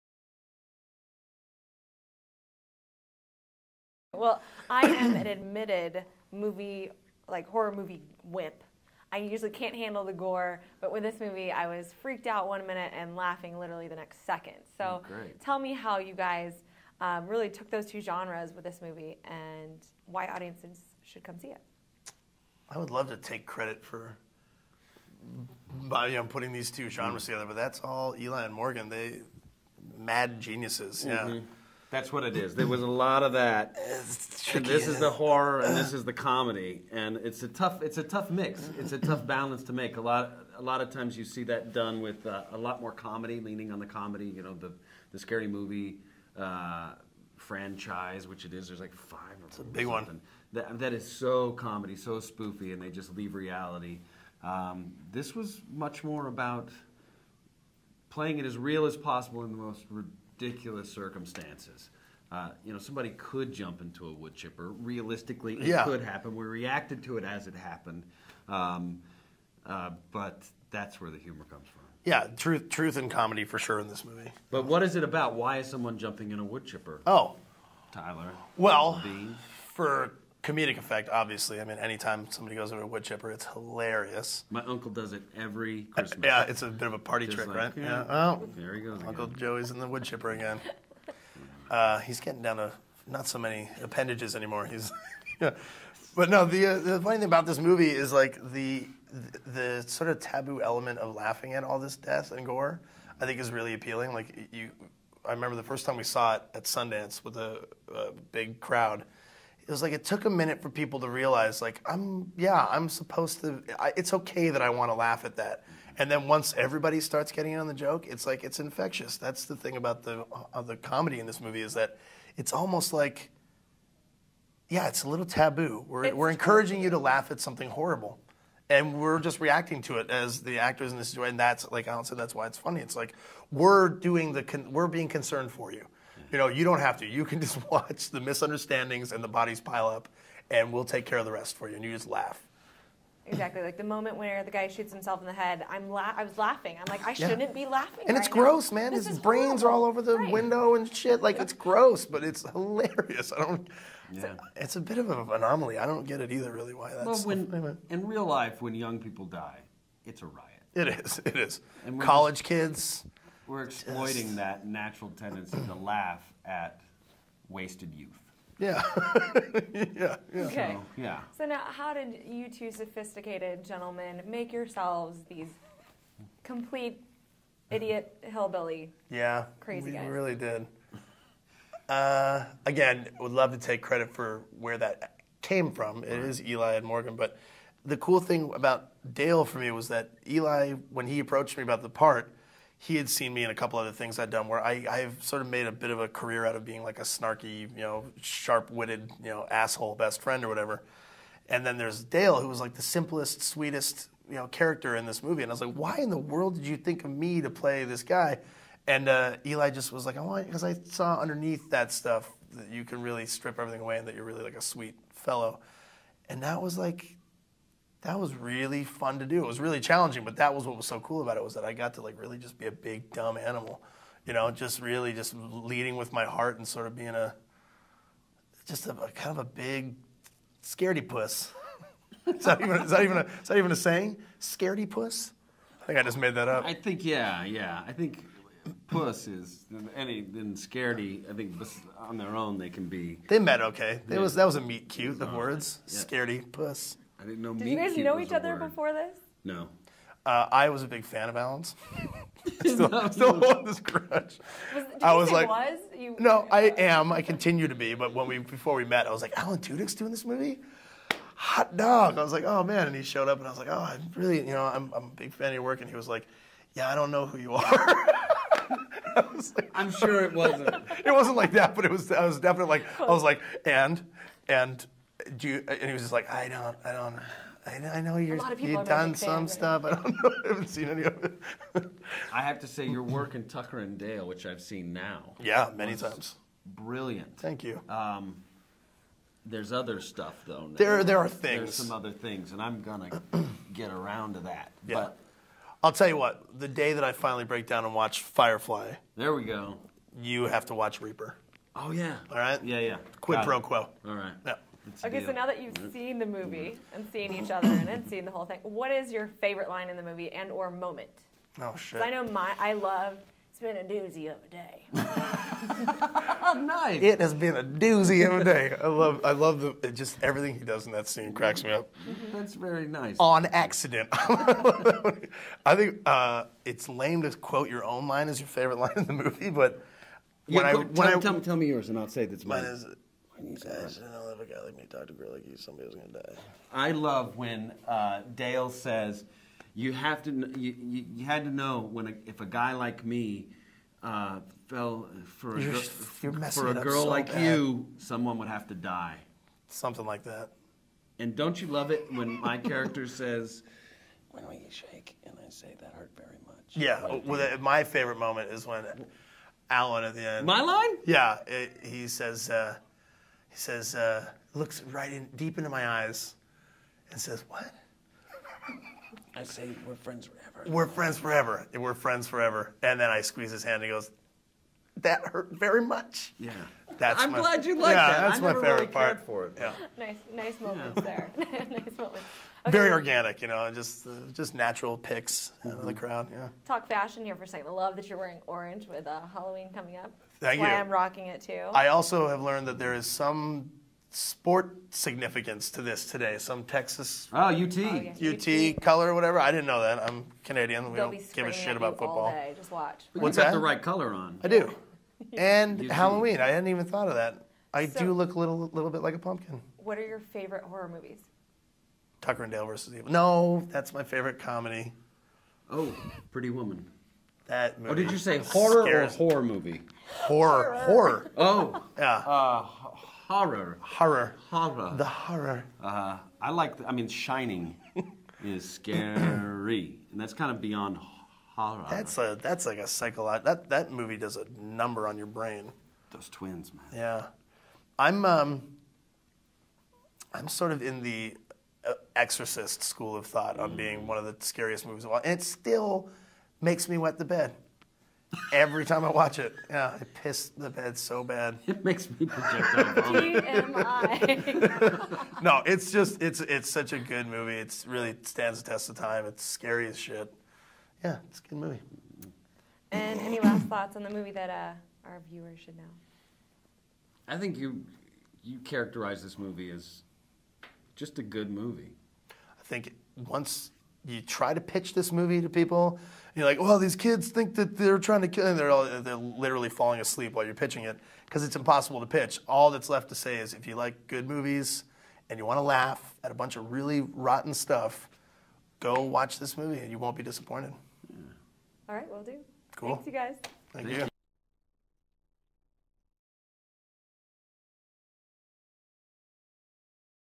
Alan Tudyk & Tyler Labine talk Tucker & Dale vs Evil
Atta Girl correspondent Cachita Hines caught up with Tyler Labine & Alan Tudyk of the new film "Tucker & Dale vs Evil" to discuss the hilarious comedy. Take a look at these two budding stars.